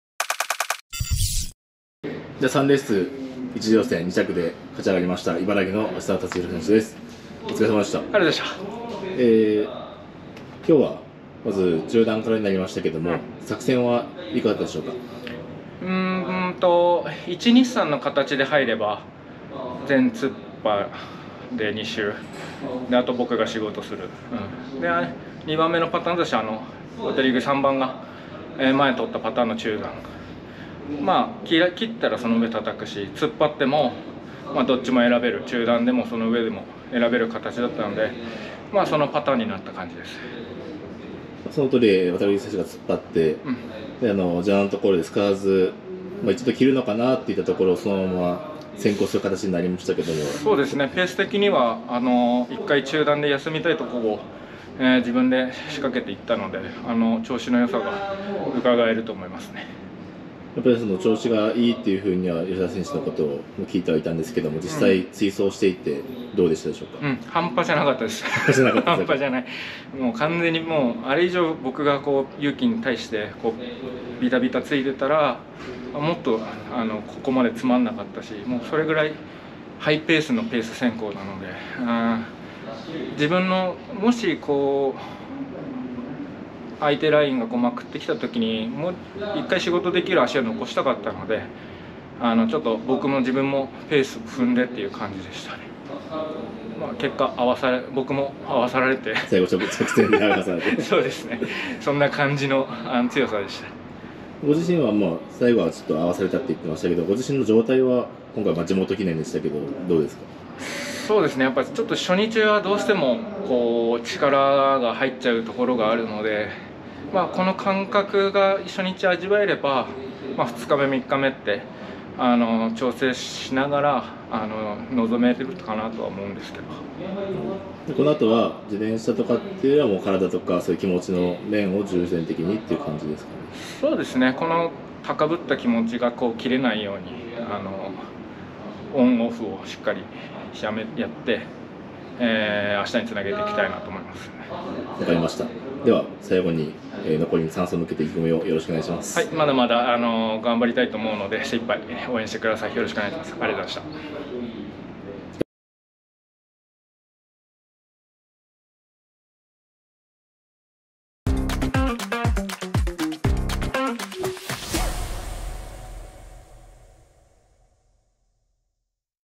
取手競輪水戸黄門賞GⅢ 芦澤辰弘(茨城・95期)初日S級一次予選 3R 2着
トリマクリでは取手競輪 水戸黄門賞GⅢのSPECIALCONTENTSを開設中！